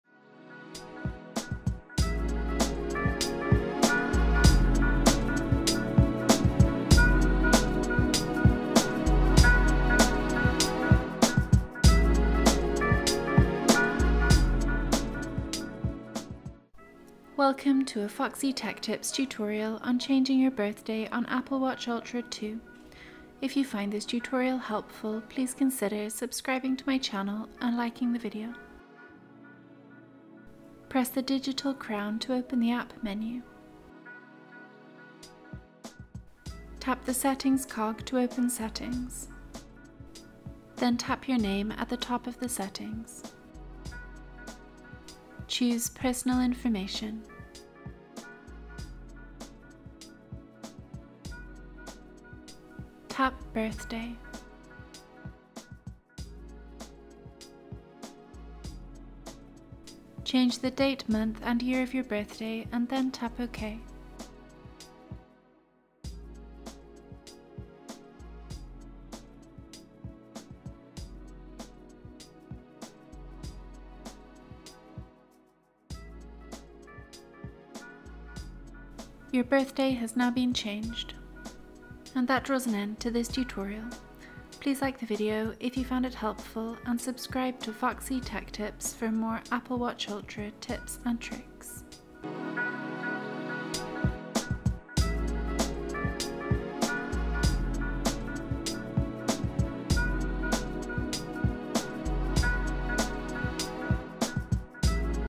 How To Change Your Birthday On Apple Watch Ultra 2 / 1
Easy to follow tutorial on changing your date of birth on an Apple Watch Ultra 2 & 1. Learn how to edit your birthday on Apple Watch Ultra now!

Steps:

Press the digital crown to open the app menu.

Tap the settings cog to open settings.

Then tap your name at the top of the settings.

Choose ‘personal information’.

Tap ‘birthday’.

Change the date, month, and year of your birthday and then tap ‘ok’.

Your birthday has now been changed.